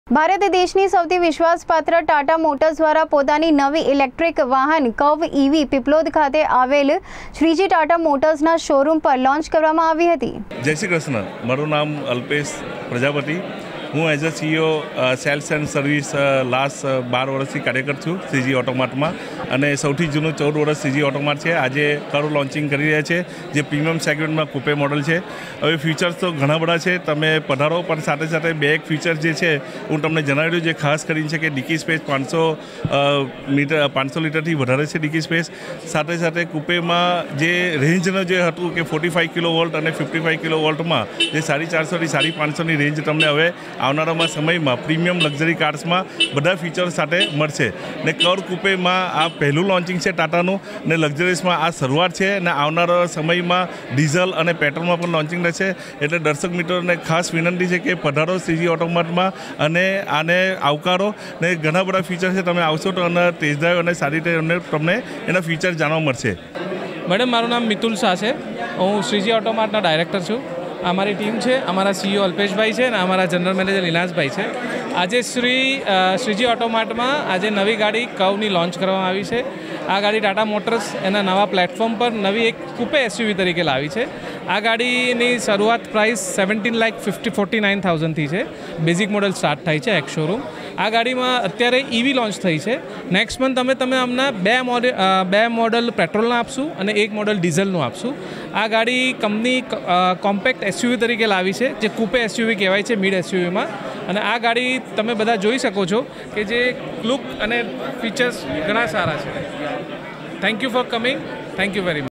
ભારત દેશની સૌથી વિશ્વાસપાત્ર ટાટા મોટર્સ દ્વારા પોતાની નવી ઇલેક્ટ્રીક વાહન curve. Ev લોન્ચ કરી.
ભારત દેશની સૌથી વિશ્વાસપાત્ર ટાટા મોટર્સ દ્વારા પોતાની નવી ઇલેક્ટ્રીક વાહન curve. Ev પીપલોદ વિસ્તારમાં આવેલી શ્રીજી ટાટા મોટર્સ ના શોરૂમ પર લોન્ચ કરી